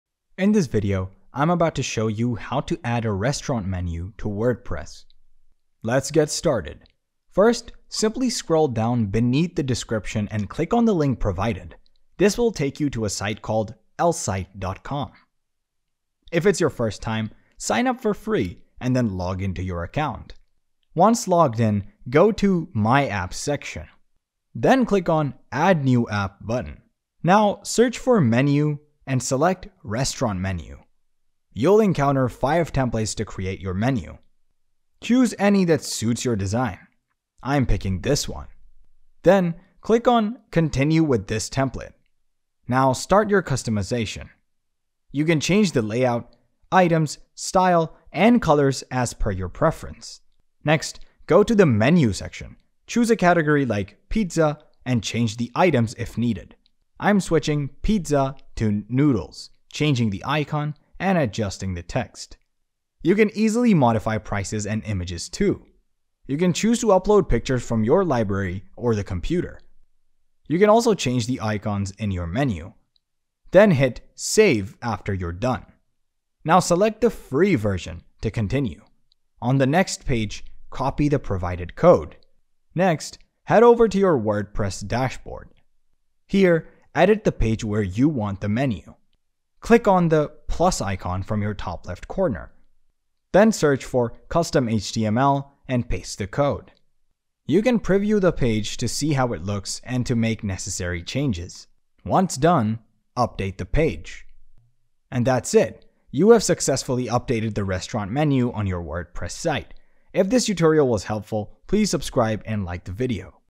How to Create a Restaurant Menu in WordPress (2024)
In this video I'll show you how you can easily add a restaurant menu to your WordPress website. Add your menu quick and easy, without any coding!